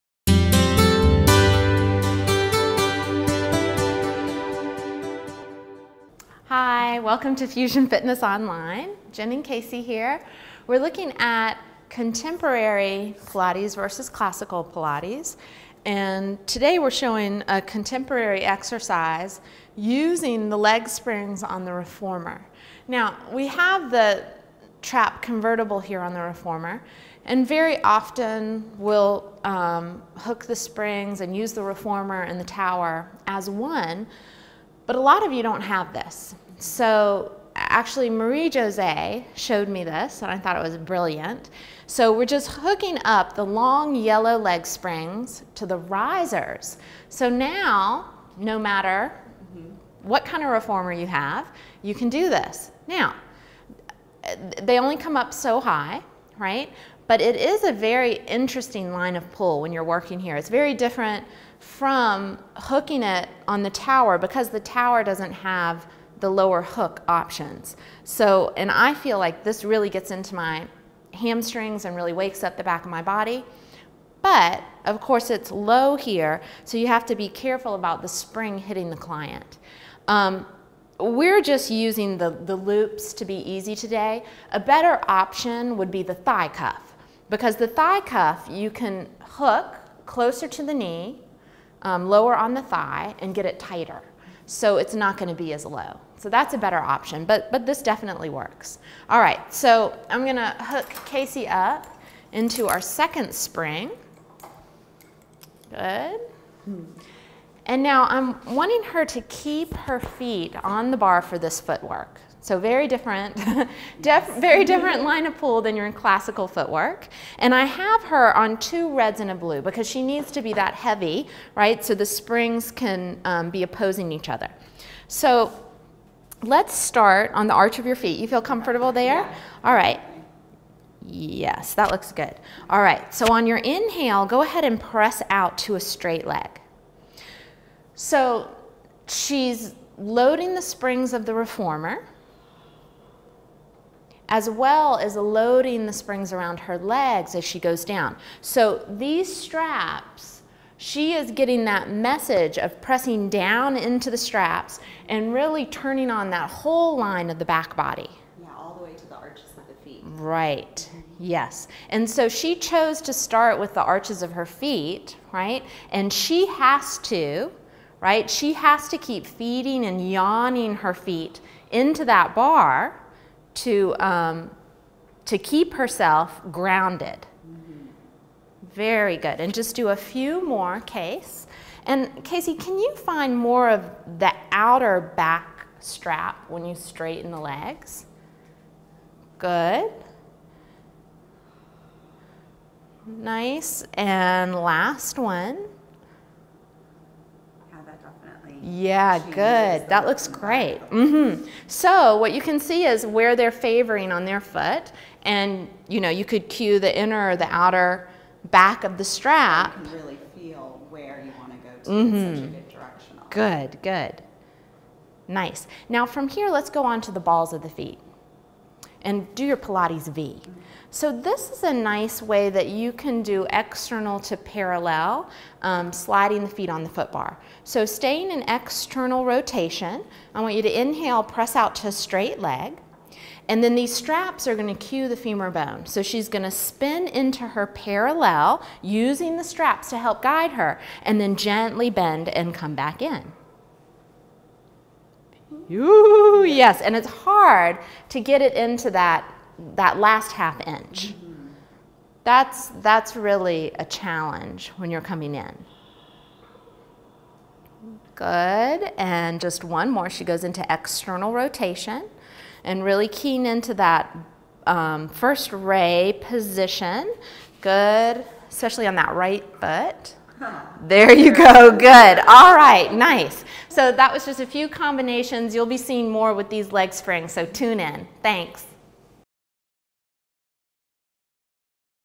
The Pilates Dork: Footwork with Springs on Risers
Watch an innovative way to wake up the back body during footwork. By adding a spring above the knee, we add another way to find contrast inside the exercise. The extra spring also helps decompress the knees, feeds the femur head deeper into the socket, and aids in the rotation of the femur bone in its socket. This is an excellent exercise for spotting misalignment in the foot, lower leg, and femur. The strap above the knee adds another floor that you can cue from, and the Spring gives the extra support the client needs to find those subtle shifts into proper alignment. The video below will show you what we mean.